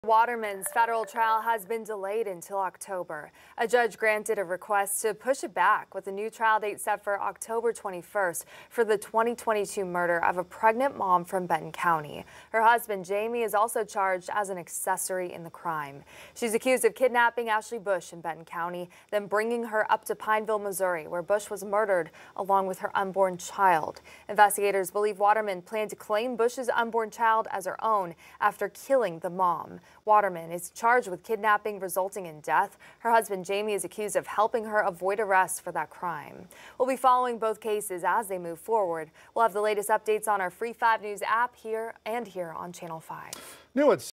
Trial delayed again for woman accused of killing pregnant Arkansas woman
Federal prosecutors are asking to seek the death penalty against Amber, according to officials.